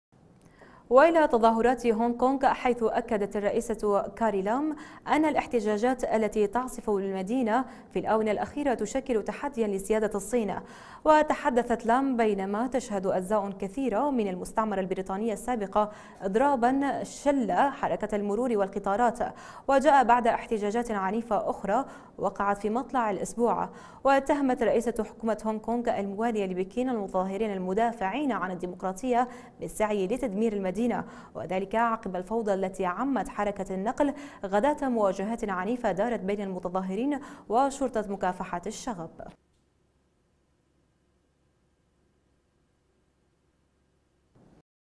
الرئيسة تؤكد أن المتظاهرين يسعون لتدمير المدينة
تظاهرات هونغ كونغ

والى تظاهرات هونغ كونغ , حيث أكدت الزعيمة كاري لام , أن الاحتجاجات التي تعصف بالمدينة في الآونة الأخيرة تدفعها إلى شفا وضع بالغ الخطورة وتشكل تحديا لسيادة الصين. وتحدثت لام , بينما تشهد أجزاء كثيرة من المستعمرة البريطانية السابقة إضرابا شل حركة المرور والقطارات، وجاء بعد احتجاجات عنيفة أخرى وقعت في مطلع الأسبوع. واتهمت رئيسة حكومة هونغ كونغ، الموالية لبكين، المتظاهرين المدافعين عن الديموقراطية بالسعي لتدمير المدينة، وذلك عقب الفوضى التي عمّت حركة النقل غداة مواجهات عنيفة دارت بين المتظاهرين وشرطة مكافحة الشغب.